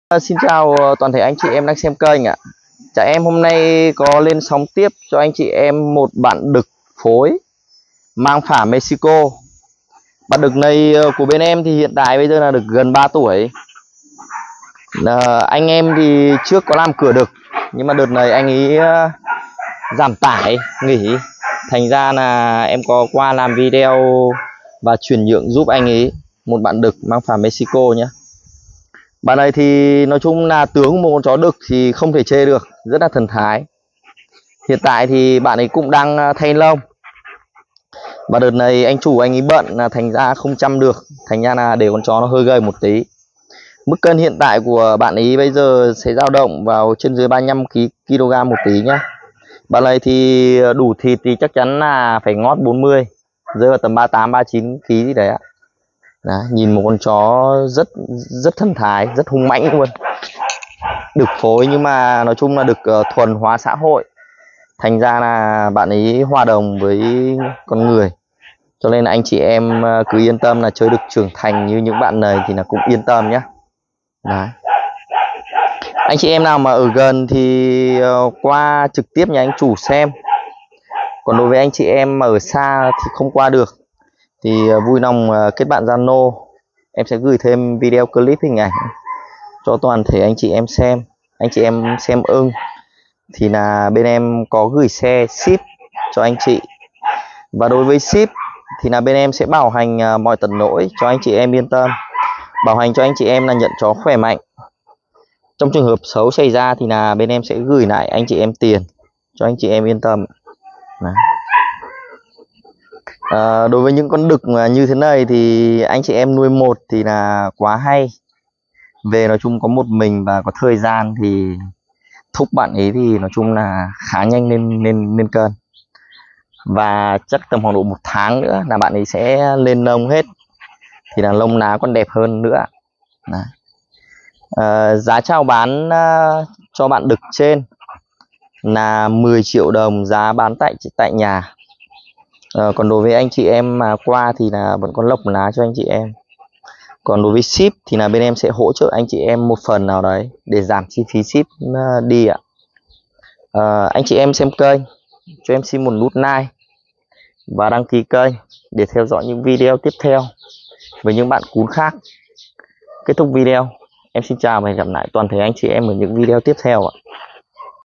đực phối mexico to cao có màu duron đậm để anh em nuôi chơi hay làm cửa đực
hiện tại bạn ấy đang thay lông và vẫn chưa được đủ thịt hẳn.